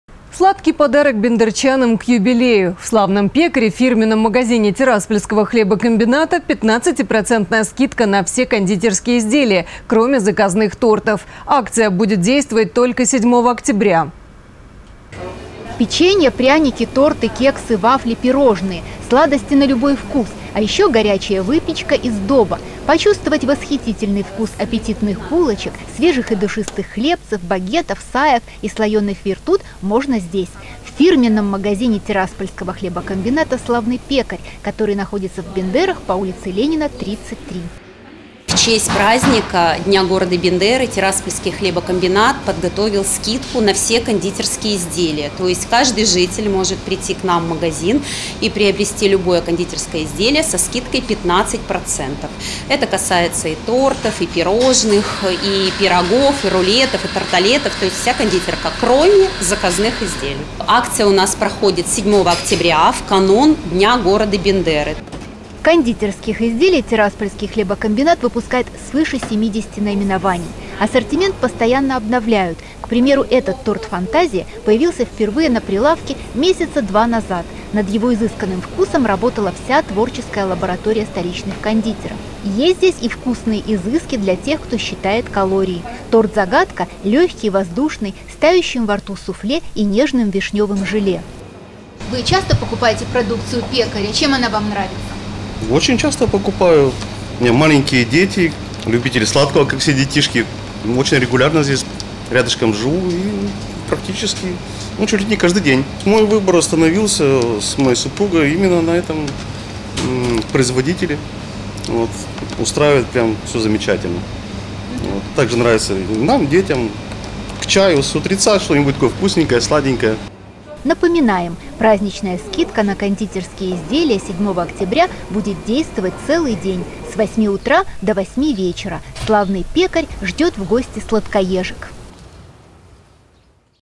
Сладкий юбилей
Сладкий подарок бендерчанам к юбилею. В «Славном пекаре» - фирменном магазине Тираспольского хлебокомбината – 15-% скидка на все кондитерские изделия, кроме заказных тортов. Акция будет действовать только 7 октября.